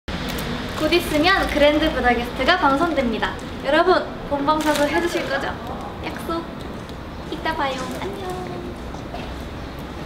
[GWSN_NOW] 오늘은 레나가 나오는 그랜드 부다개스트 하는 날!
오늘은 공원소녀 레나가 출연하는 [JTBC2 그랜드부다개스트] 하는 날! 그루들 잊지말고 오늘 밤 8시 본방사수 해주세요!😘

Today is GWSN Lena 's [JTBC2 GrandBudaguest] broadcasting day! Don't forget to watch the show tonight at 8PM, Groo!😘